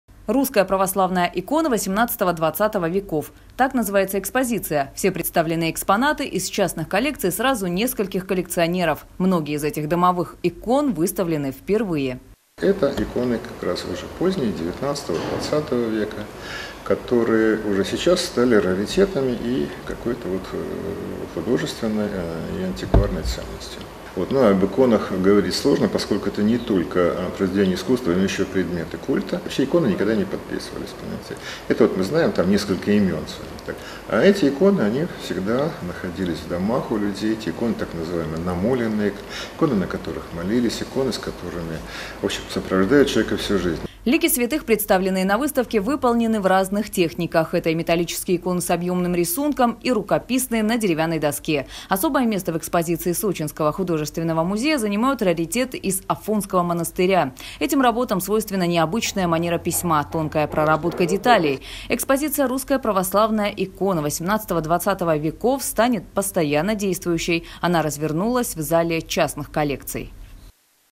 В Сочи открылась выставка православных икон. Новости Эфкате
Экспозиция развернулась в Сочинском Художественном музее.

«Русская православная икона 18-20 веков». Так называется экспозиция. Все представленные экспонаты - из частных коллекций сразу нескольких коллекционеров. Многие из этих домовых икон выставлены впервые.